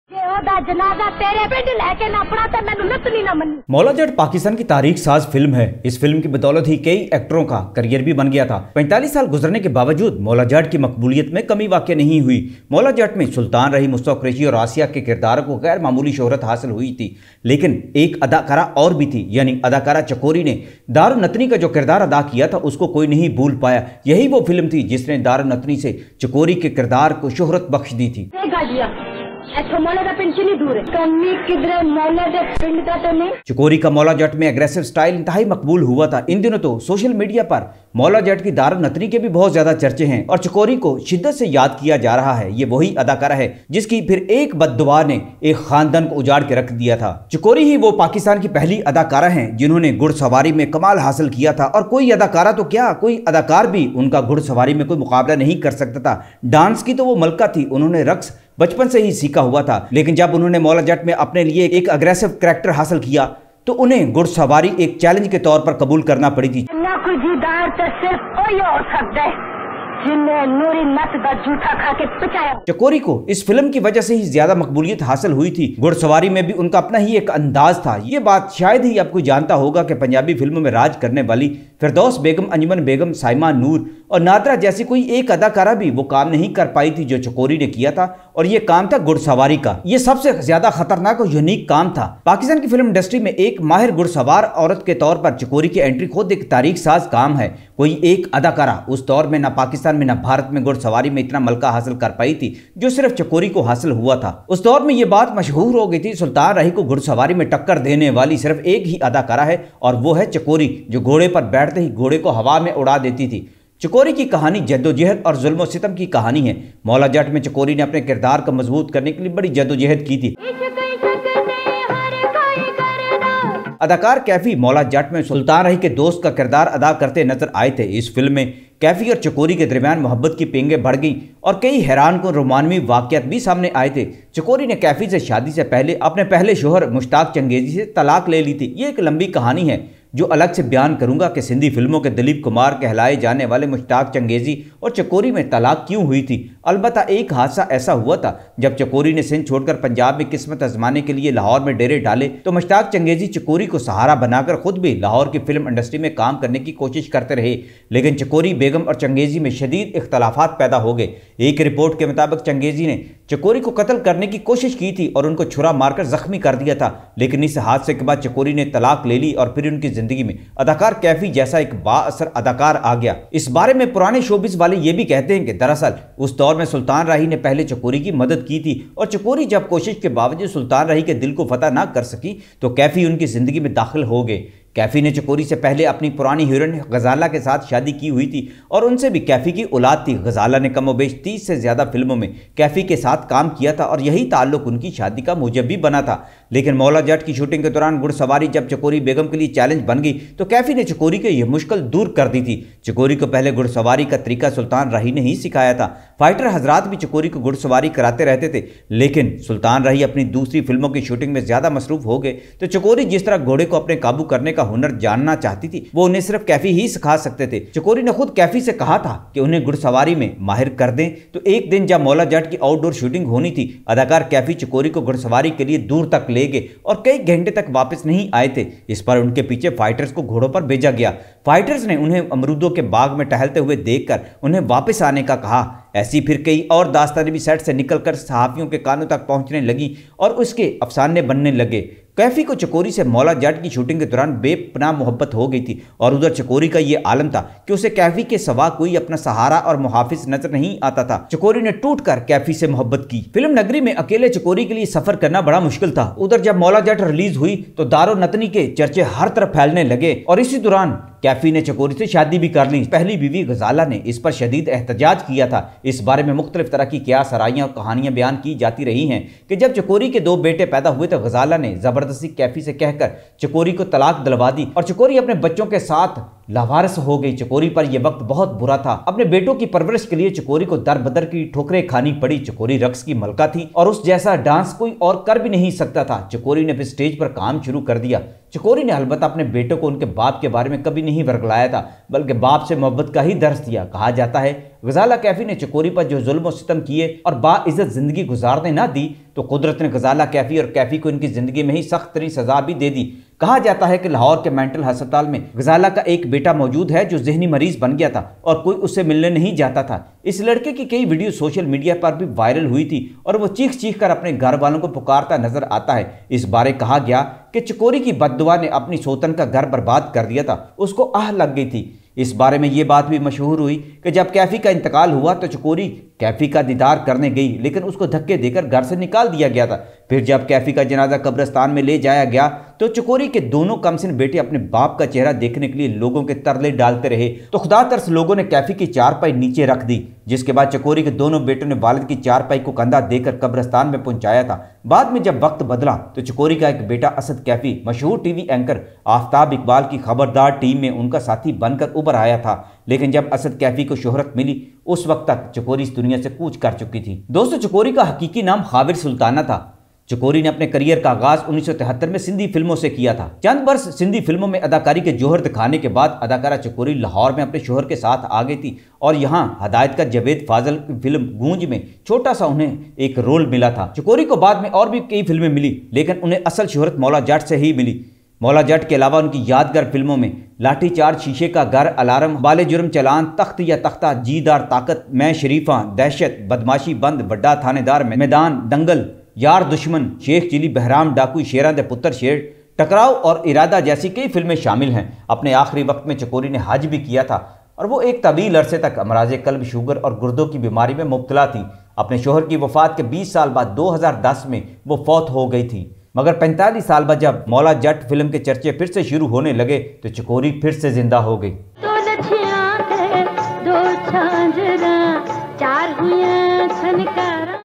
The Sad Story Of Actress Chakori|Maula Jutt| Inqalabi Videos
Actress Chakori, who played the character of Daro  natni in the Pakistani film Maula Jutt, was an extremely brave woman. In this video of Inqalabi, it is revealed that actress Chakori learned horse riding from Sultan Rahi  and actor Kaifee. After the shooting of Maula Jutt, Chakori Married to Great Pakistani actor Kaifee. Actor Kaifee's elder brother Inayat Hussain Bhatti was a very famous folk and film singer and actor. Actor Kaifee was also married to actress Ghazala  Kaifee but then divorced Chakori. Chikori get divorced Sindhi film,s Dilip Kumar mushtaq changezi  and married Kaifee. Chakori acted in many Sindhi Punjabi and Urdu films. Chakori was the only actress who was skilled in horse riding. Chakori got immense fame with the role of Daro Natni in the movie Maula Jutt. Chakori's son Asad Kaifee is a part of Aftab Iqbal's team in the popular show Khabardar. The film Maula juttt is a successful and historical film .chakori was very famous Dancer in lollywood.Chakori   did many dramas for stage and theater with Actor Afzal Ahmed and other pakistani actors.
مولا جٹ کی دارو نتنی
اداکارہ چکوری